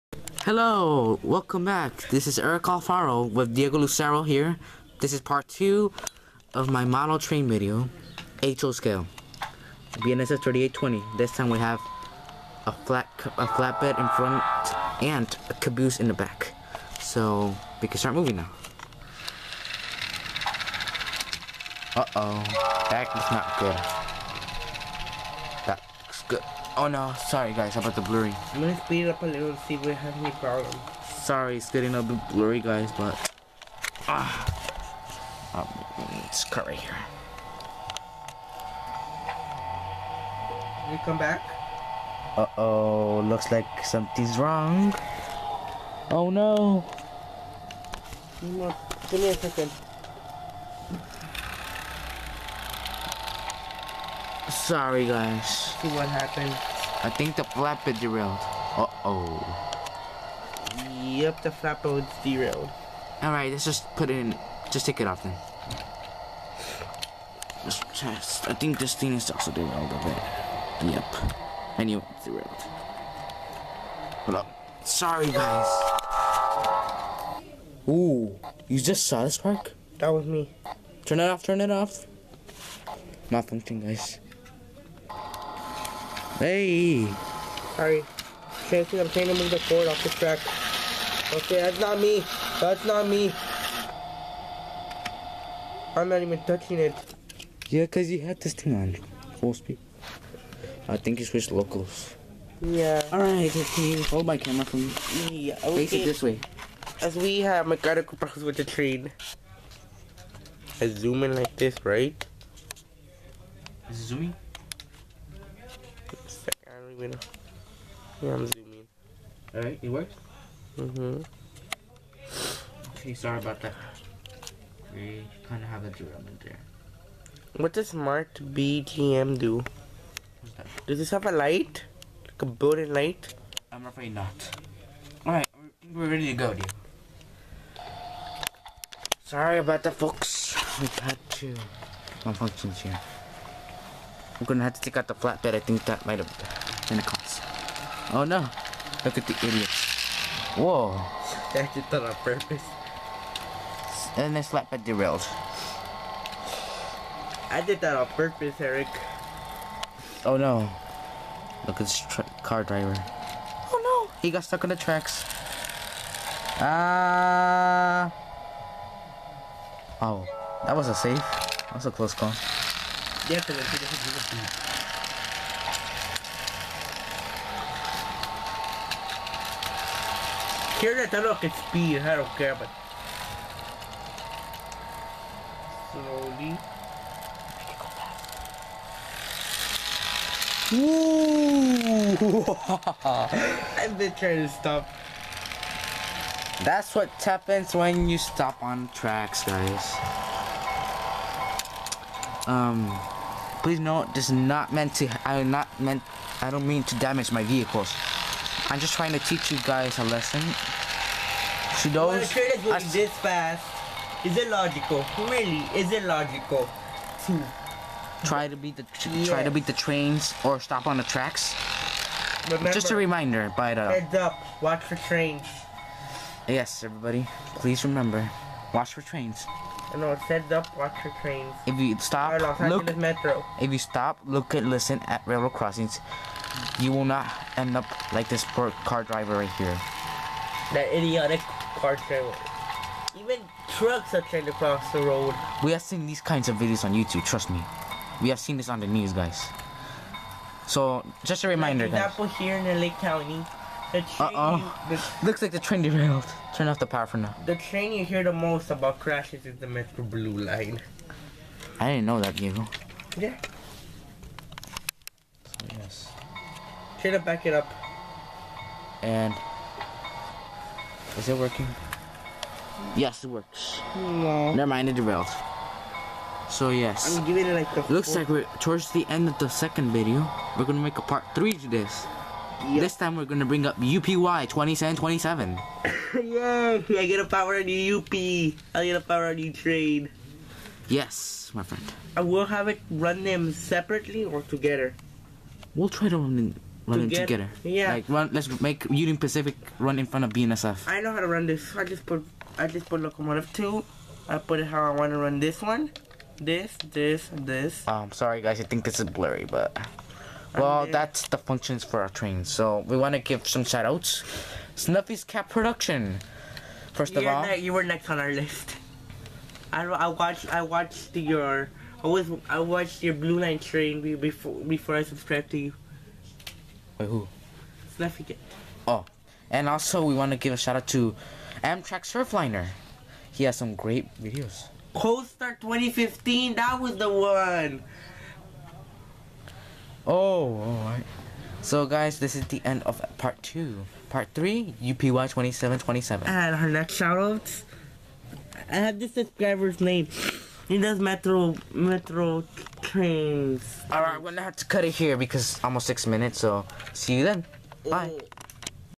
Erik and Diego's Model Railroad Series - Episode 2
Here's episode 2 of the series featuring BNSF 3820 and an idiot car driver (model) with an important lesson at the end. Enjoy!